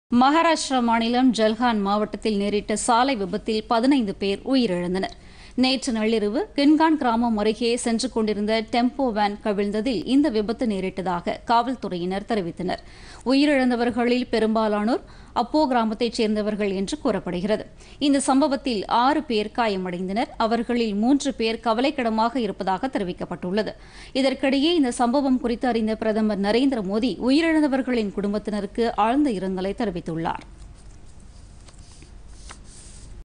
சாலை விபத்தில் 15 பேர் உயிரிழப்புPodhigaiTamilNews
மகாராஷ்ட்ரா மாநிலம் ஜல்கான் மாவட்டத்தில் ஏற்பட்ட பயங்கர சாலை விபத்தில் 15 பேர் உயிரிழப்பு - குடியரசுத் தலைவர், பிரதமர்  இரங்கல்.